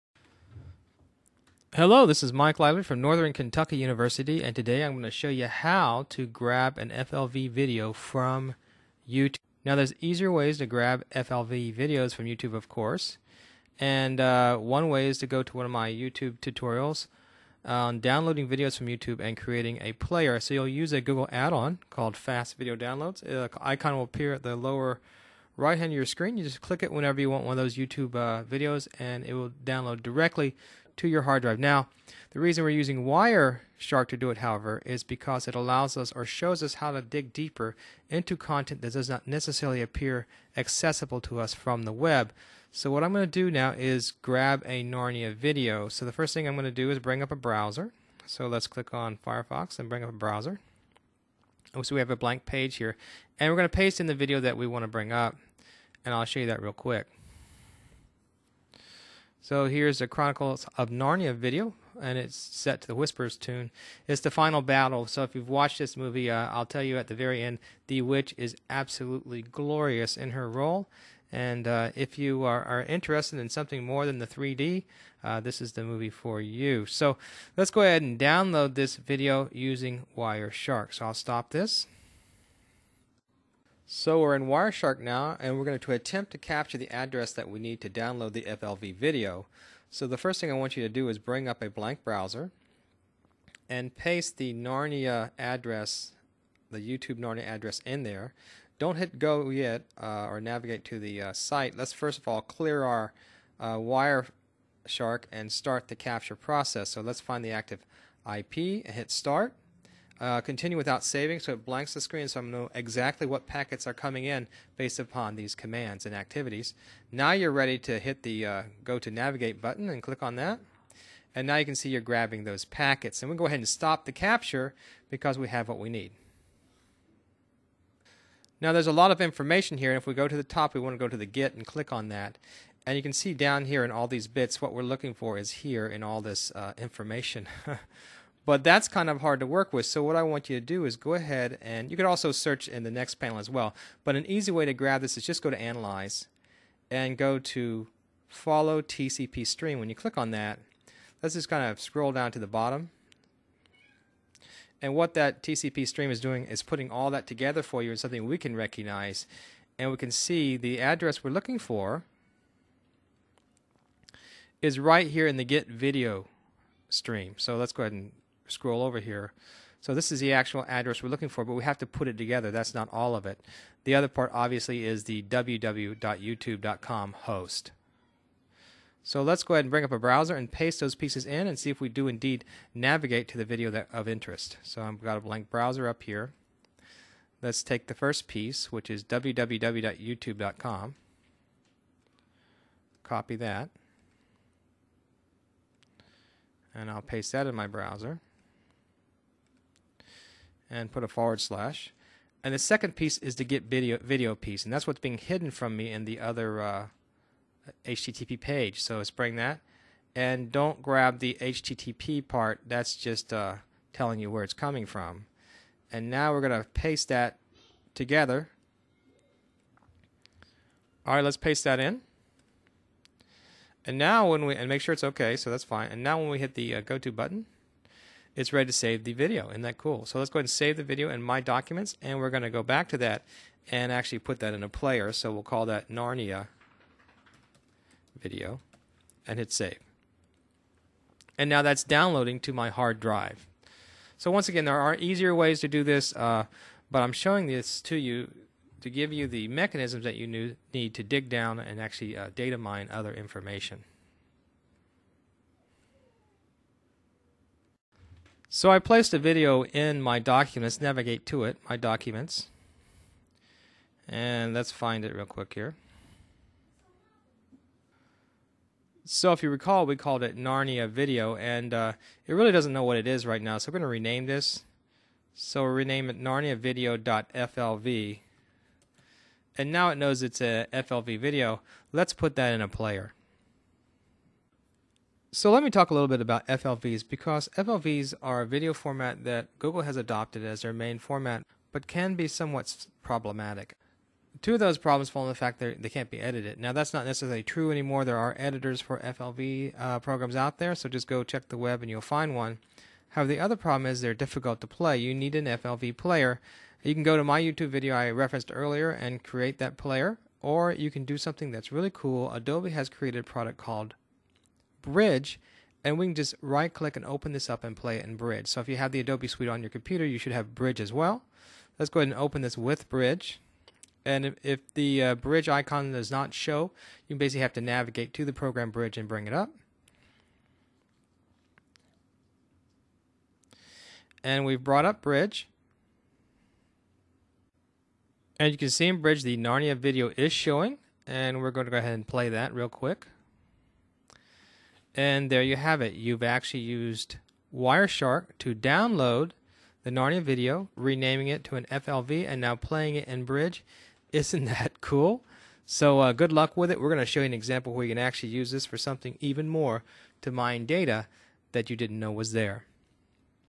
Data Mining using Wireshark (Part 3 of 3)
Part 3 of this series demonstrates how to mine data to grab an FLV video from Youtube. In addition, Adobe Bridge is demonstrated.

This tutorial was created by Mike Lively of Northern Kentucky University.